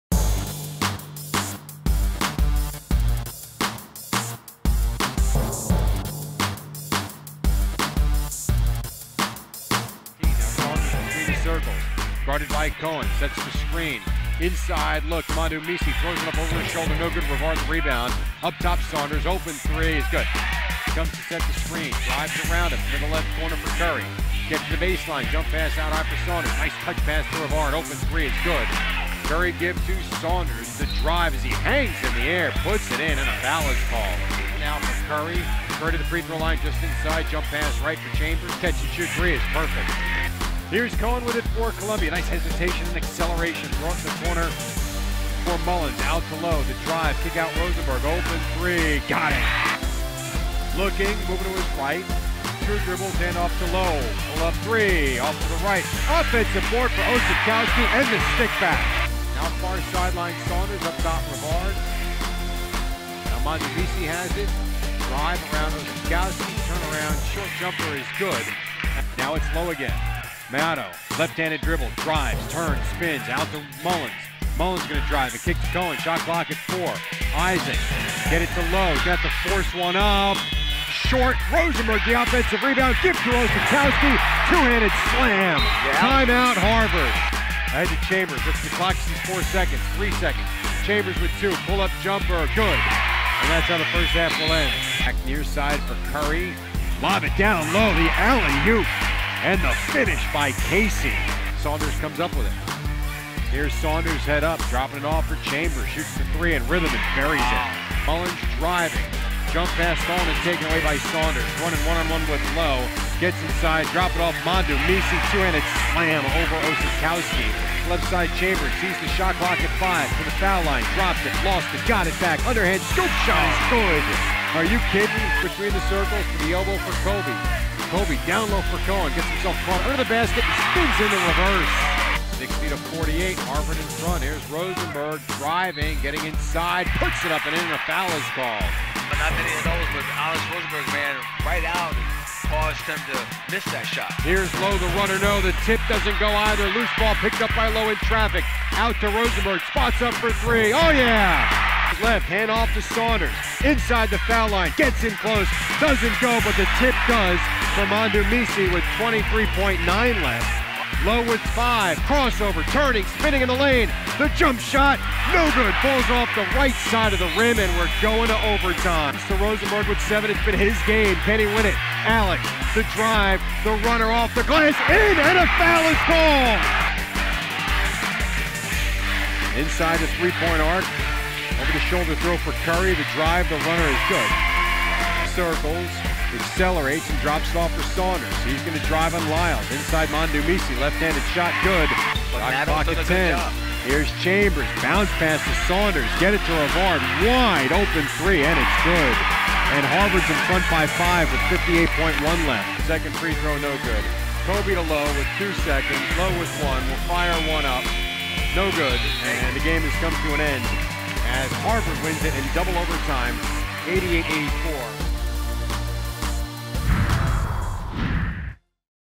Highlights: Men's Basketball Earns 2OT Victory at Columbia, 88-84
The Crimson picked up their second overtime victory of the year as Siyani Chambers, Steve Moundou-Missi and Wesley Saunders powered the team to an 88-84 win at Columbia.